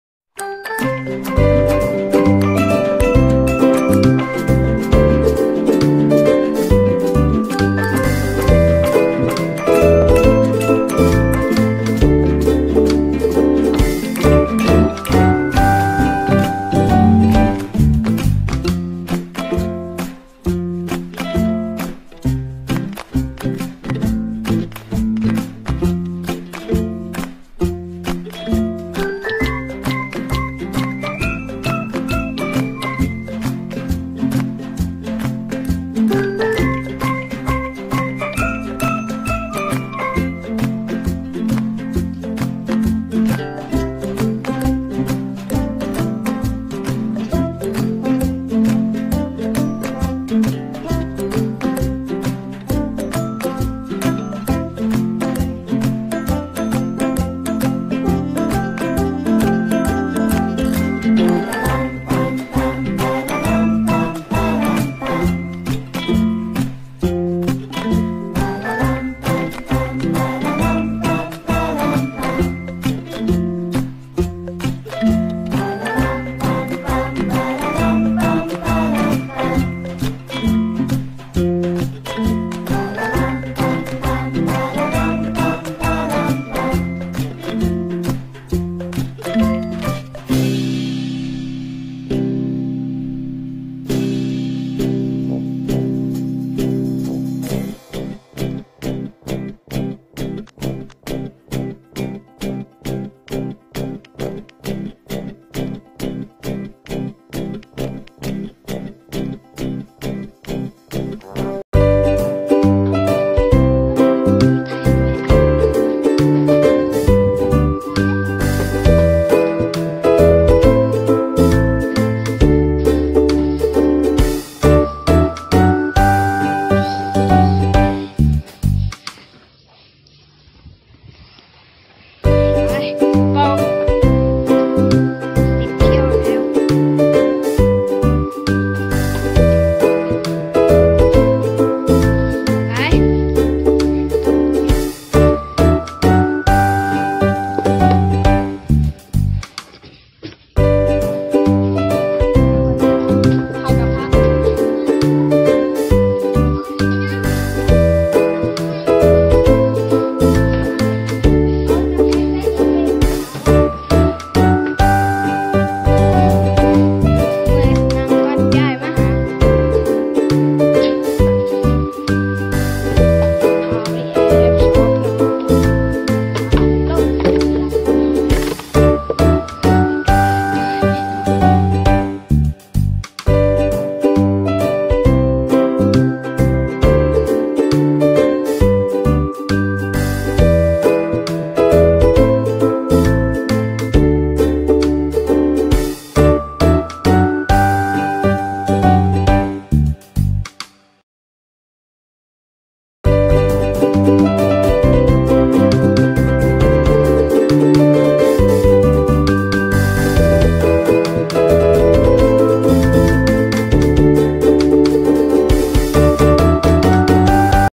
รีโนเวสบ้านให้คุณเเม่ไกล้สำเร็จเเล้วค่ะ
ด้วยหยาดเหงื่อของเราเอง